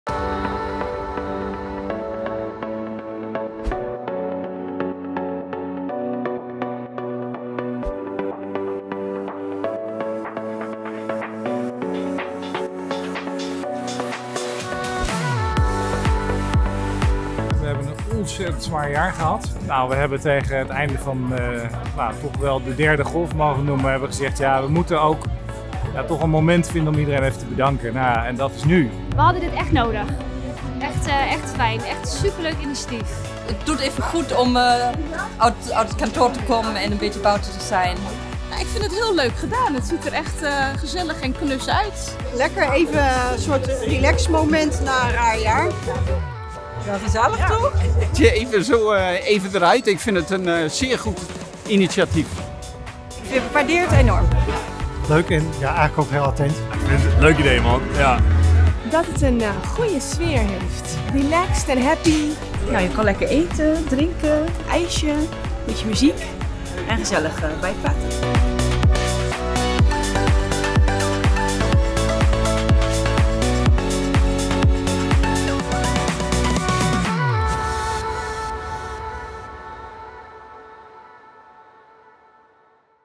Oh what a year!
Wat een jaar hebben we achter de rug. Om alle medewerkers te bedanken voor de tomeloze inzet, organiseerde het Erasmus MC het evenement Oh, what a year. Twee pleinen op het ziekenhuisterrein werden tijdelijk omgetoverd tot een festivalterrein, inclusief foodtrucks en zomerse tunes. Een gezellige week!